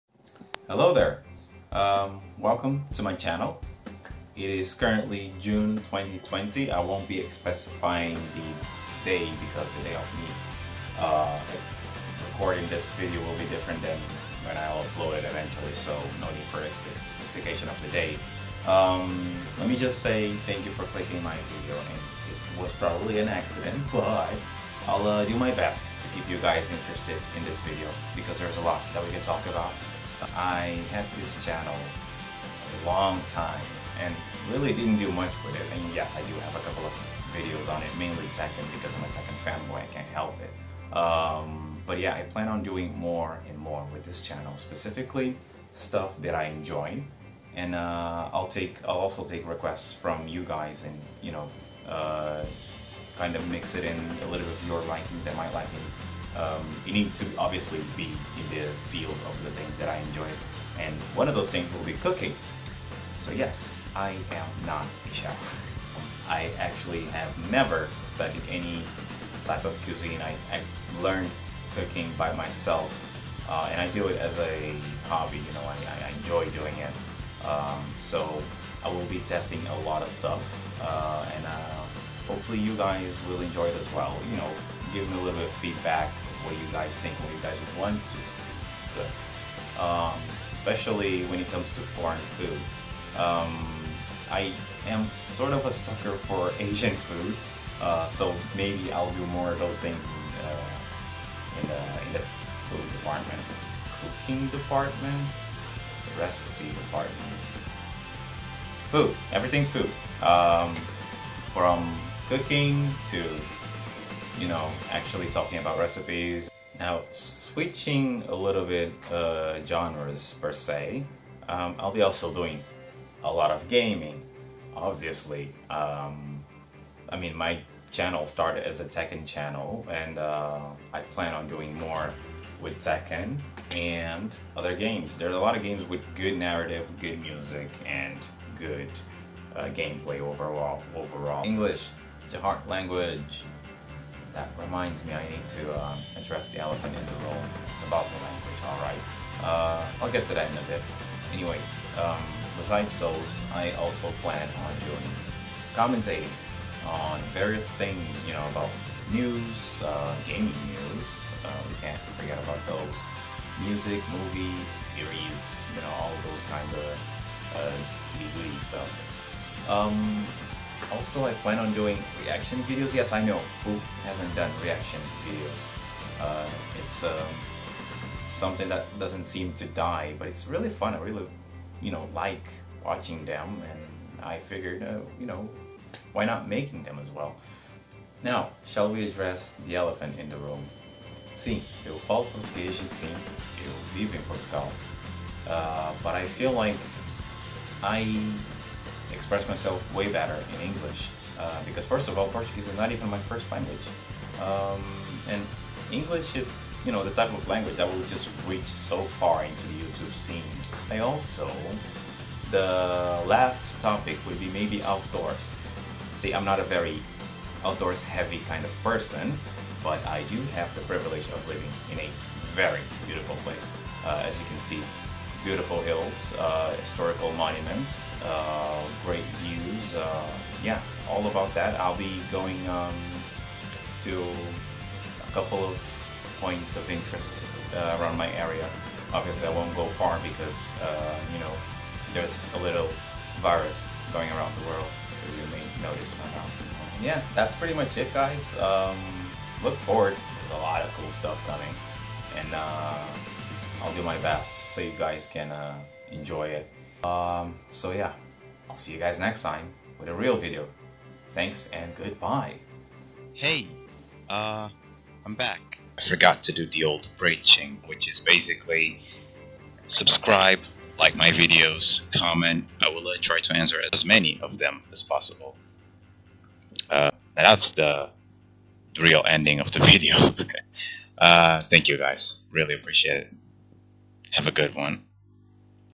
Hello There, I Am New (Kinda)!
After so long, I finally decided to give a proper use to this channel. In this video, I will be explaining what will be the future of the channel and the type of content you can expect from it!